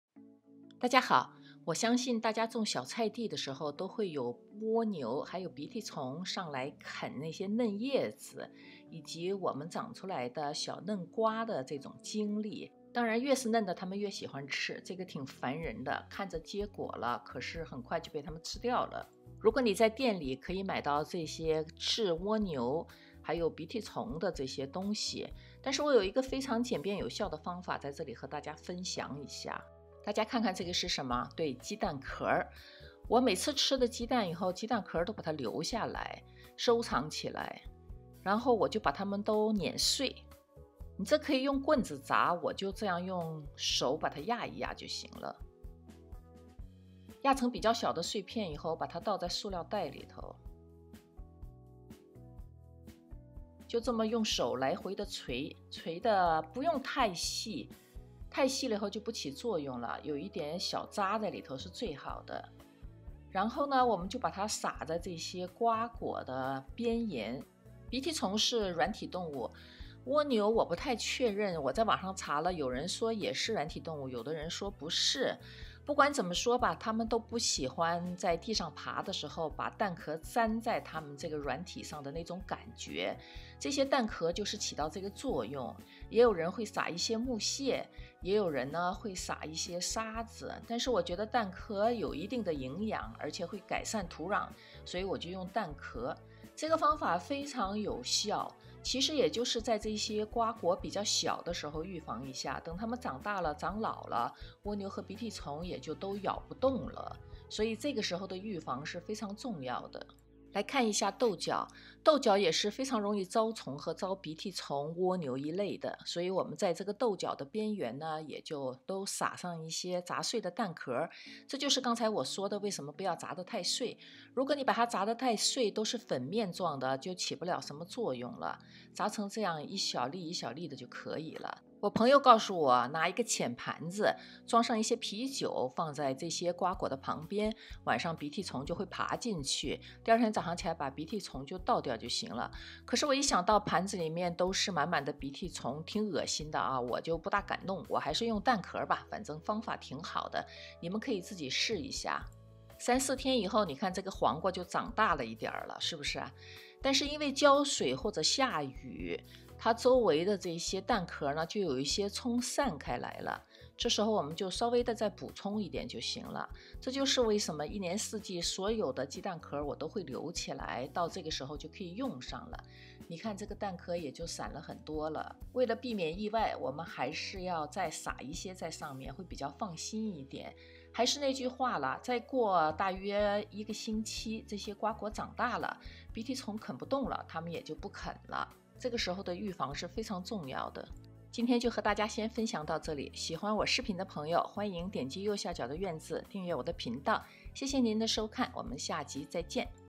【彬彬有院】种•小菜地/小花园如何防治蜗牛和鼻涕虫------简易的鸡蛋壳法
相信大家都有过这样的经历，辛苦种下的菜，好好地刚结出来的嫩果实就被鼻涕虫或者蜗牛给啃了。我给大家提供一个简单易行的办法可以解决这个问题。